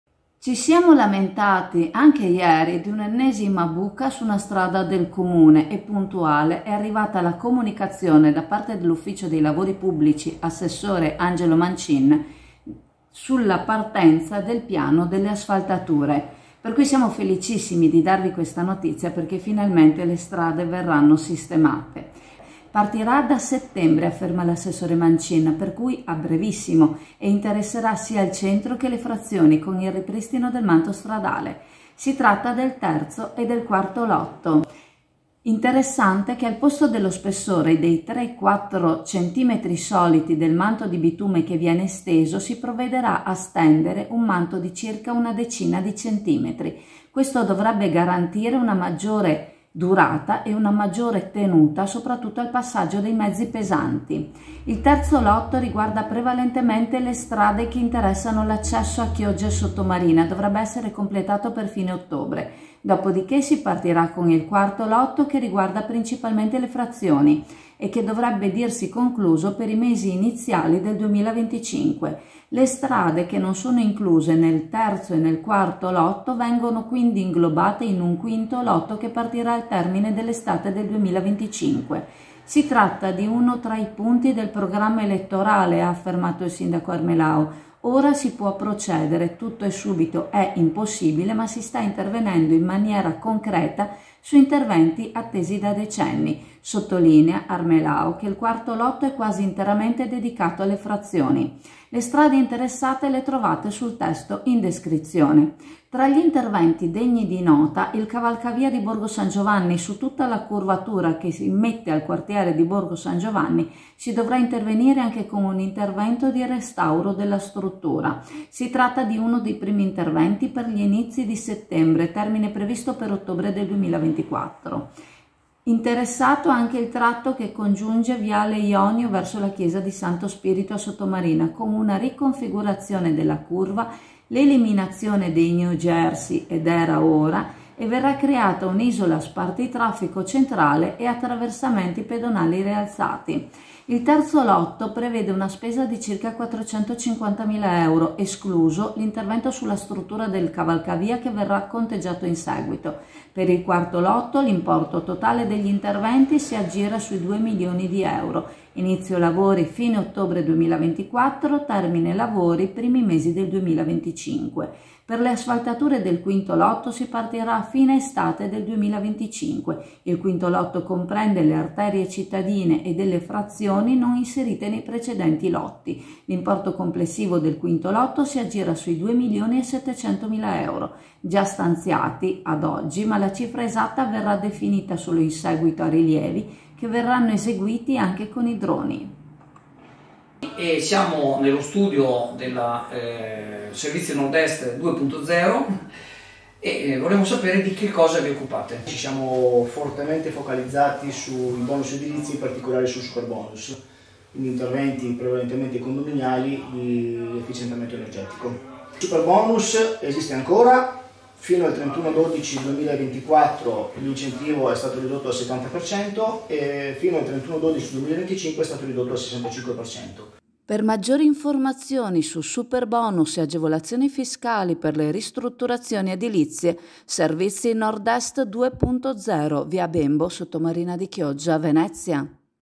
FINALMENTE TUTTE LE STRADE - CENTRO E FRAZIONI - SARANNO ASFALTATE
Chioggia, 20 agosto 2024

Ci siamo lamentati anche ieri di un’ennesima buca su una strada del comune e puntuale è arrivata la comunicazione da parte dell’Ufficio LLPP, assessore Angelo Mancin, della partenza del piano delle asfaltature.
Partirà da settembre, , afferma l’assessore Mancin, per cui a brevissimo, e interesserà sia il centro che le frazioni con il ripristino del manto stradale. Si tratta del 3° e del 4° lotto.
Interessante che al posto dello spessore di 3 o 4 cm soliti del manto di bitume, si provvederà a stendere un manto di circa una decina di centimetri. Questo dovrebbe garantire una durata maggiore e una maggior tenuta soprattutto al passaggio dei mezzi pesanti.
Il 3° lotto riguarda prevalentemente le strade che interessano l'accesso a Chioggia e Sottomarina e dovrebbe essere completato per fine ottobre. Dopo di che si partirà con il 4° lotto che riguarda principalmente le frazioni e che dovrebbe dirsi concluso per i mesi iniziali del 2025.
Le strade che non sono incluse in questi lotti, vengono inglobate nel 5° lotto che partirà al termine dell’estate 2025
Si tratta di uno tra i punti del programma elettorale, ha affermato il sindaco Armelao, ora si può procedere, tutto e subito è impossibile, ma si sta intervenendo in maniera concreta su interventi attesi da decenni. Sottolinea, Armelao, che il 4° lotto è quasi interamente dedicato alle frazioni.
Le strade interessate le trovate sul testo in descrizione.
Tra gli interventi degli di nota il cavalcavia di Borgo San Giovanni; su tutta la curvatura che immette al quartiere di Borgo San Giovanni, si dovrà intervenire anche con un intervento di restauro della struttura.
Si tratta di uno dei primi interventi, per gli inizi di settembre, termine previsto per ottobre 2024 circa.
Interessato anche il tratto che congiunge Viale Ionio verso la Chiesa dello Spirito Santo, a Sottomarina, con riconfigurazione della curva e l'eliminazione dei new jersey, verrà creata un'isola spartitraffico centrale e attraversamenti pedonali rialzati.
Il terzo lotto prevede una spesa di circa 450mila euro, escluso l’intervento sulla struttura del cavalcavia che verrà riconteggiato in seguito.
Per il 4° lotto l’importo totale degli interventi si aggira sui 2 milioni di euro. Inizio lavori: fine ottobre 2024 per terminare nei primi mesi del 2025.

Pel le asfaltature del 5° lotto si partirà a fine estate 2025. Il 5^ lotto comprende le arterie cittadine e delle frazioni non inserite nei precedenti lotti. L’importo complessivo del 5° lotto si aggira sui 2 milioni e 700 mila euro, già stanziati ad oggi ma la cifra esatta verrà definita solo in seguito ai rilievi che verranno eseguiti anche con droni.

LOTTO 3

• Dalla rotonda davanti ospedale direzione ovest, entrambi i sensi di marcia tutte le corsie, con spessori variabili, fino all’altezza del cavalcaferrovia di Borgo San  Giovanni;
• Svincoli da cavalcaferrovia svolta direzione Venezia-Padova, fino all’ingresso in SS 309 Romea;
• Da sopra cavalcavia della ss 309, direzione Ravenna, per tutta la curvatura fino all’ingresso SS 309 Romea;
• Da sopra cavalcavia ss 309 Romea, della strada per il porto (direzione sud), tutta la curvatura direzione Ravenna.

LOTTO 4

L’Amministrazione ha in programma la sistemazione e la bitumatura di alcune strade comunali da eseguirsi dopo rilievo stradale e restituzione grafica, con distinzione delle varie aree e tipologie di intervento.
Le vie che verranno sistemate sono le seguenti:

VIA AUSONIA a Sottomarina
VIA ESPERIA a Sottomarina
VIALE IONIO/SANTO SPIRITO
VIA CAVOUR a Chioggia
VIA DEL BOSCO a Cavanella
VIA LUNGO ADIGE a Cavanella (tratto da SS Romea a Ferrovia)
VIA CANNONI (a tratti) a S. Anna
VIA PEGORINA a S. Anna
VIA CORNARO a Cà Bianca
VIA CANAL DI VALLE (2 laterali)
VIALE BRENTA (tratto da Bariga a via San Marco)
VIA OCEANIA a Isola Verde
VIA ATLANTICO a Isola Verde
VIA LUNGO BRENTA da Romea a via San Giuseppe
VIA SAN GIUSEPPE (a tratti) Ca’ Lino
VIA GARZETTA a Valli
PIOVINI (parziale)
VIALE VENETO ovest